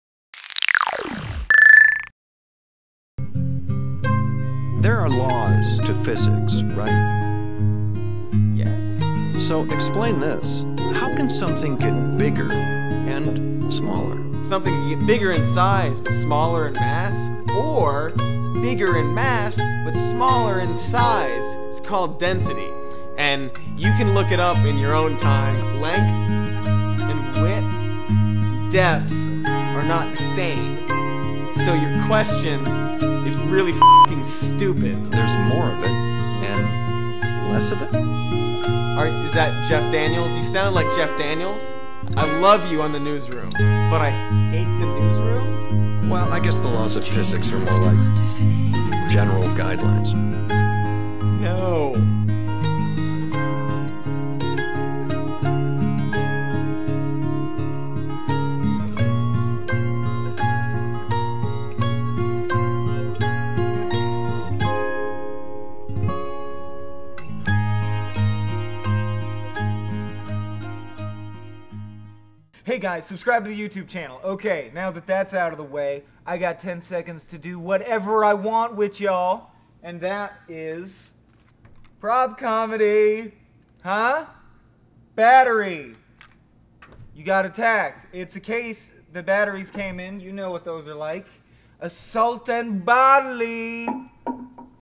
A Science Lesson For Whoever Wrote the New iPhone 5 Ad
If you haven't seen the new iPhone 5 ads, and have a basic understanding of geometry, get ready to be baffled.

Make sure you watch the end of the video for a special message from Swaim.